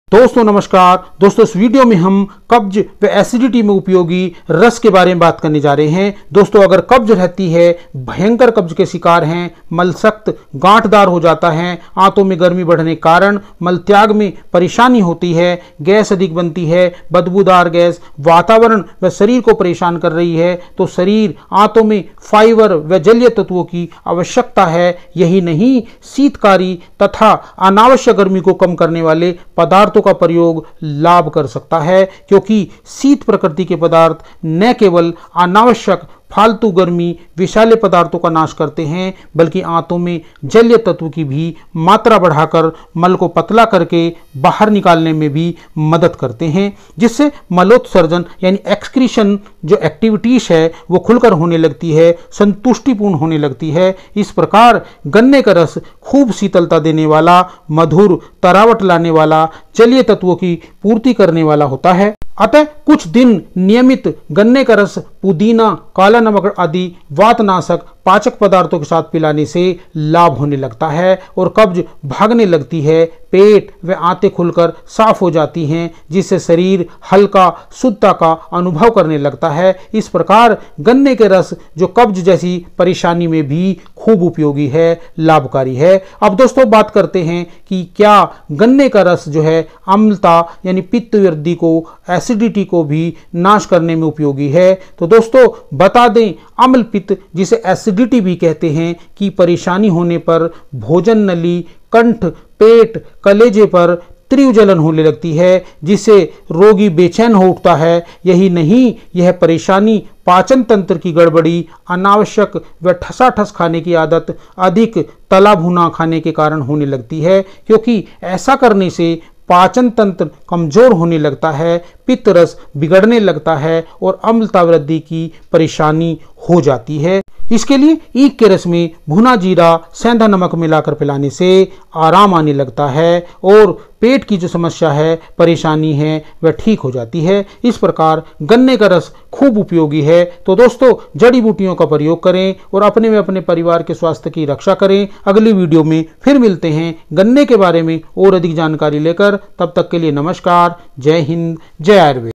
useful in constipation and acidity | कब्ज में उपयोगी प्रयोग | एसिडिटी में उपयोगी प्रयोग
इस वीडियो में अम्लपित्त जिसे एसिडिटी कहते हैं तथा constipation यानी आंतों में मल का सूखना, मल की गांठे बनना , मल खुलकर नहीं आना, कब्ज़ होना आदि परेशानियों में उपयोगी जूस के बारे में बात करने जा रहे हैं।  यह रस  प्राकृतिक तरीके से, बिना हानि पहुंचाए, धीरे-धीरे रोग का निदान कर देता है । तभी तो विद्वानों ने इसके गुणों की खूब चर्चा की है। यही नहीं इसके पौधे के जूस तथा विभिन्न उत्पादों का सारी दुनिया में खूब प्रयोग किया जाता है। तो दोस्तों वीडियो को पूरा देखें और वीडियो में दी गई जानकारी प्राप्त करें । दोस्तों बताए गए प्रयोगों ,जड़ी बूटियों का प्रयोग करें, चिकित्सक की सलाह से करें और अपने व अपने परिवार के स्वास्थ्य की रक्षा करें । अगली वीडियो में फिर मिलते हैं एक और अद्भुत जड़ी बूटी के बारे में जानकारी लेकर । तब तक के लिए नमस्कार। जय हिंद ।
जय आयुर्वेद।